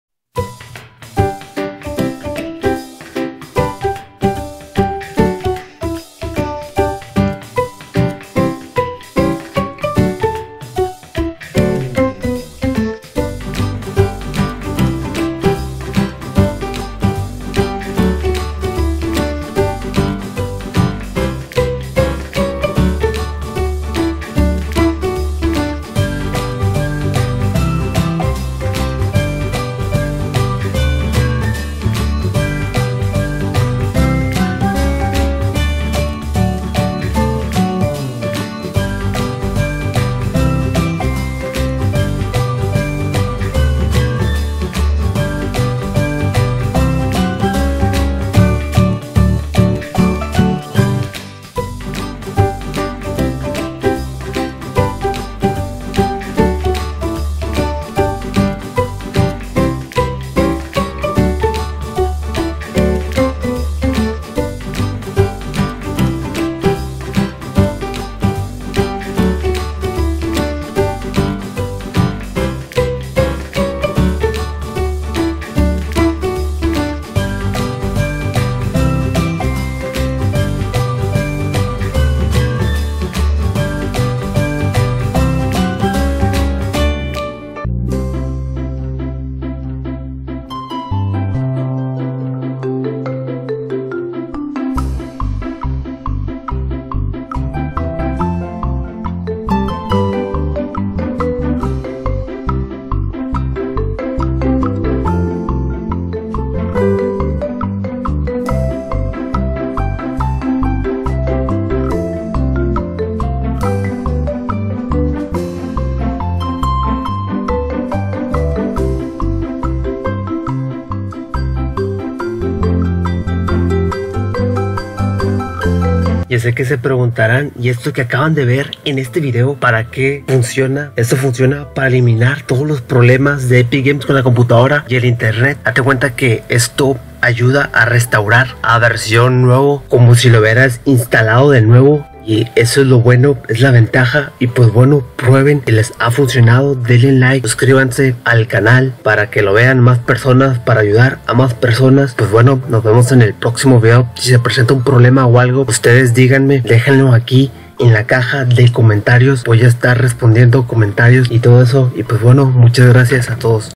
Crasheo FORTNITE Solución Temporada 5 Error crashed Application Crash Detected Diciembre 100% 2020
En este video les muestro como eliminar el problema que se presenta que se abre un cuadro de que el juego esta crasheado, por ahi he visto que en esta temporada hay personas que se les presentan estas situaciones..
The application has crashed and will now close, we apologize for the inconvenience
Diciembre 2020.